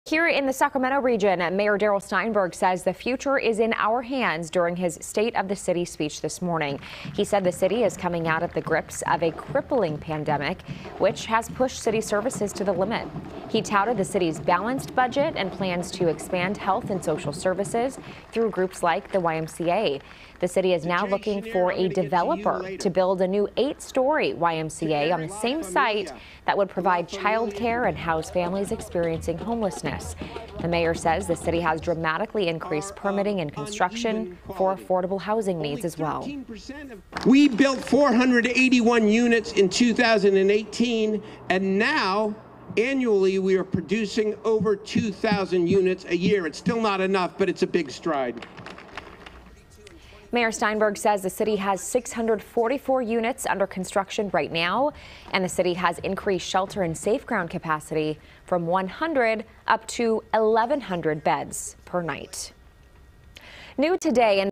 Sacramento Mayor Darrell Steinberg Gives His State Of The City Address
Today, Sacramento Mayor Darrell Steinberg gave his State of The City address where he touted the city's balanced budget.